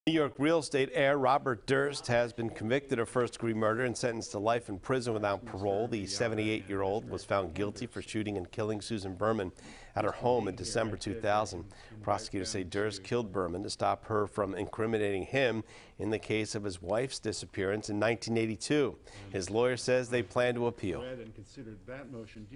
Robert Durst sentenced to life in prison for murdering friend Susan Berman
New York real estate heir Robert Durst was sentenced Thursday to life in prison without chance of parole for the murder of his best friend more that two decades ago.